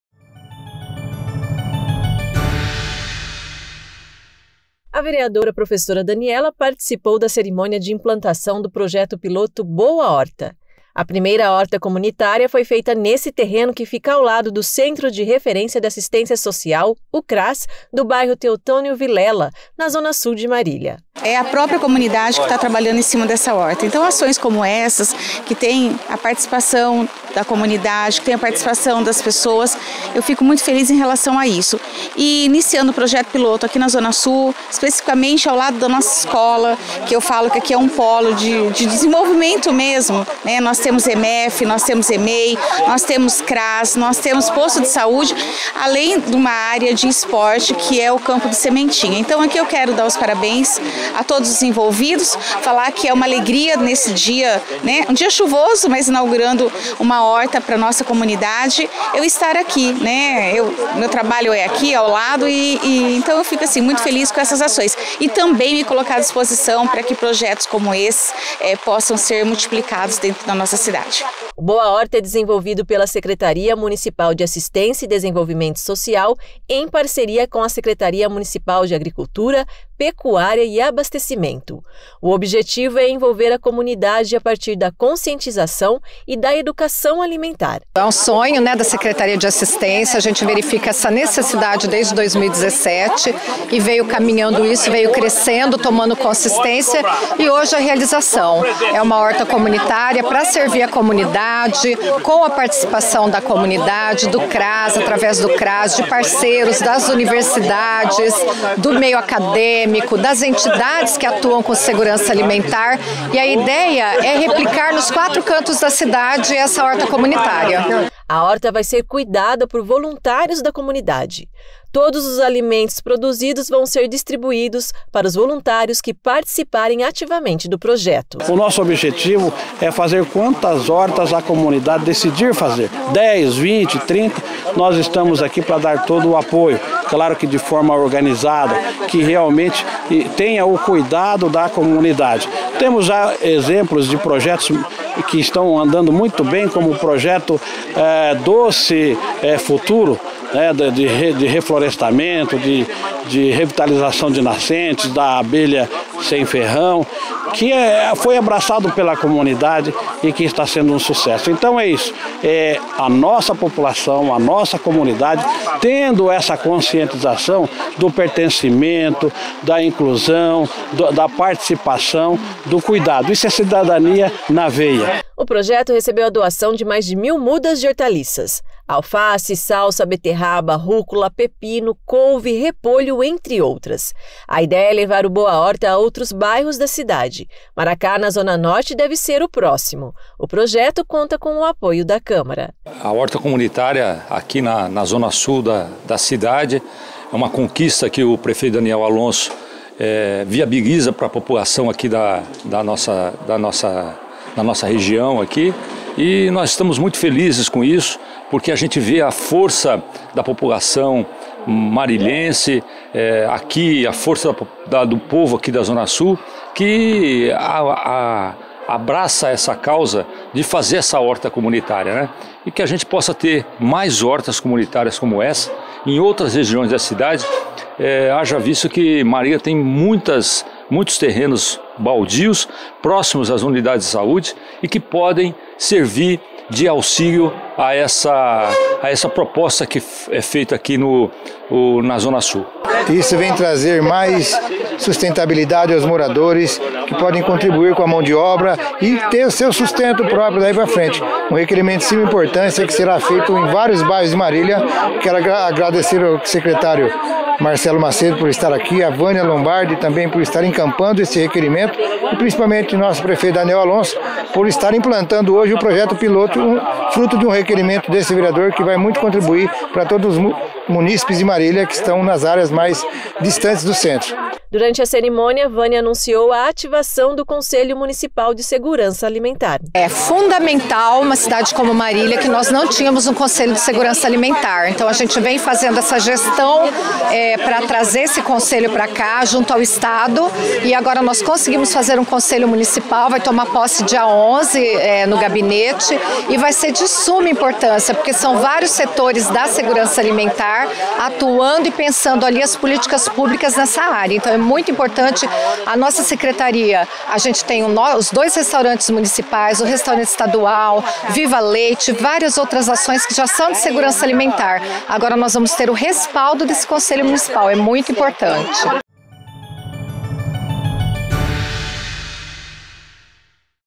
PROJETO PILOTO ‘BOA HORTA’ É IMPLANTADO NO TEOTÔNIO VILELA
Os vereadores Professora Daniela, Marcos Rezende e Evandro Galete participaram da cerimônia de implantação do projeto piloto “Boa Horta” na tarde desta terça, 09. O projeto piloto “Boa Horta” tem como objetivo principal gerar segurança alimentar para as famílias que serão beneficiadas com os alimentos e aproximar a população das questões ambientais, além de fortalecer ainda mais a agricultura familiar.